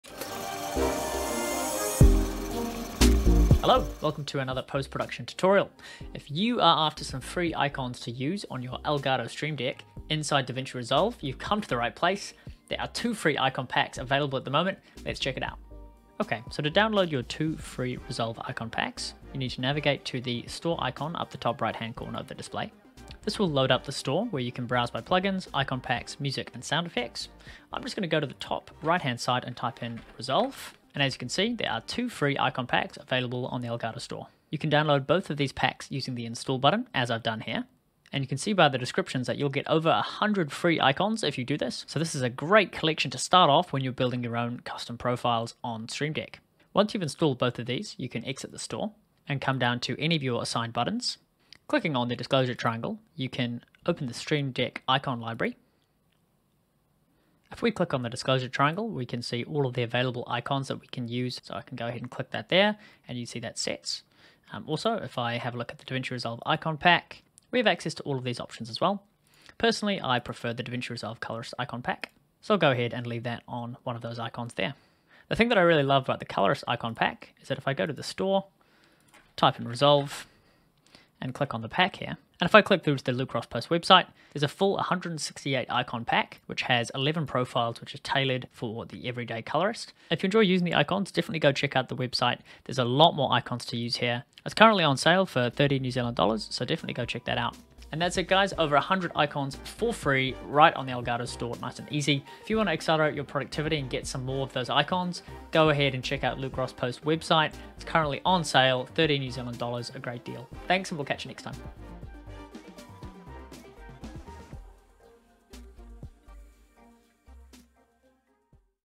100+ FREE Resolve Icons | Elgato Stream Deck Store
WHAT'S INCLUDED
- 232 PNG icons (72x72px).
- Dynamic, visual icon layouts with labels
- 11x individual, pre-programmed Stream Deck profiles.
- 1x Stream Deck master profiles backup.
- 1x DaVinci Resolve shortcut key preset profile.
- 1x Shortcut PDF Guide.
- 1x ReadMe document.

COMPATABILITY
- Compatible with Windows and MacOS.
- Shortcut keys made for DaVinci Resolve.
- Designed for the Elgato Stream Deck (not included).

CHAPTERS
00:00 Intro
00:16 Elgato Store
01:22 Colourist Icon Pack